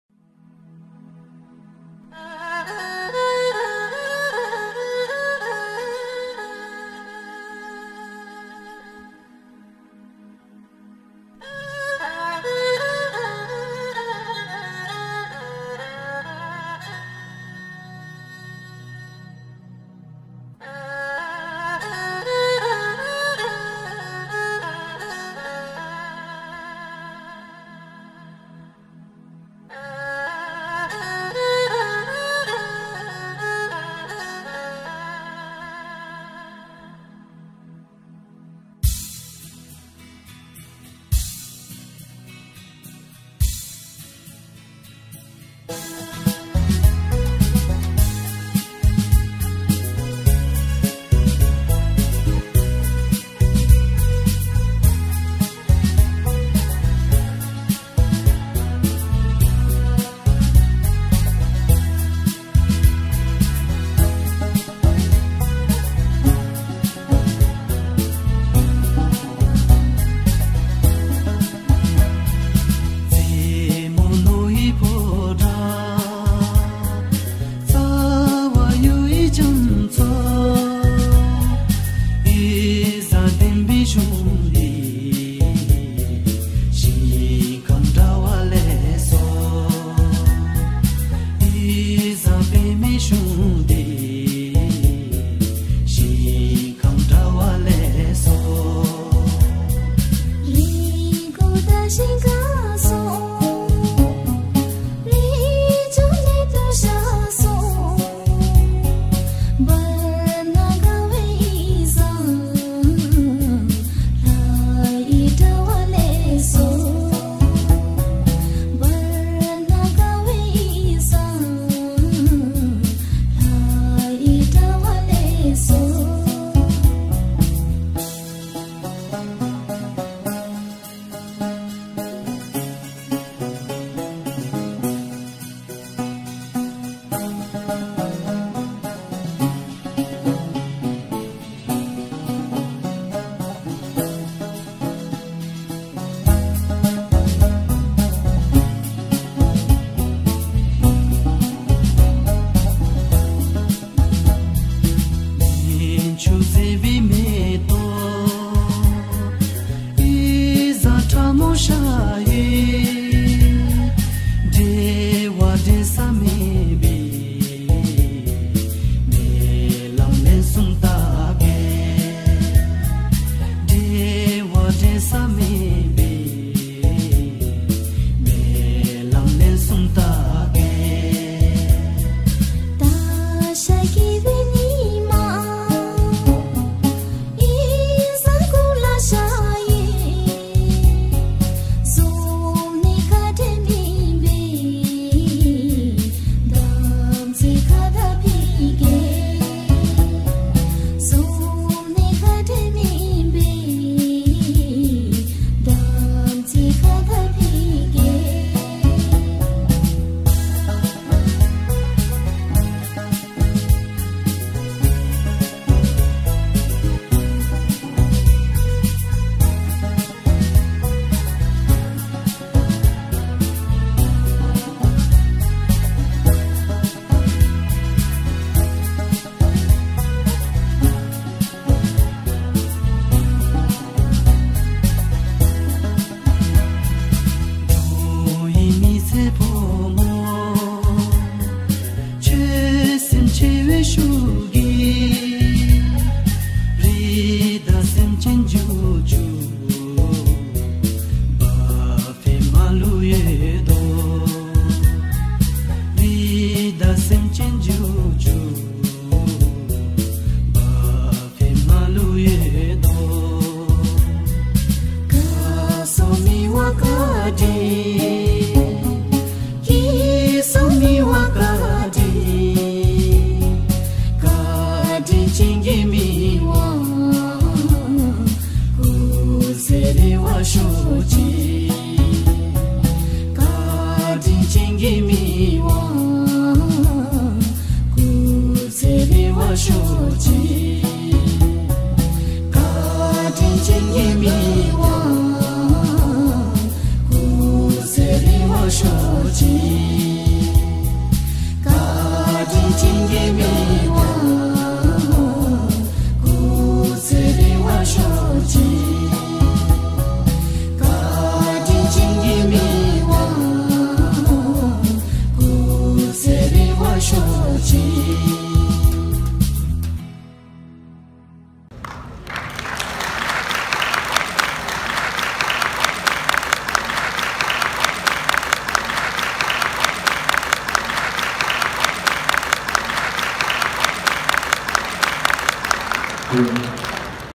Boedra by Marriot company on 112th Bhutan National Day in Kuwait
Bhutanese celebrated National Day in Kuwait Crown Plaza. 17/12/2019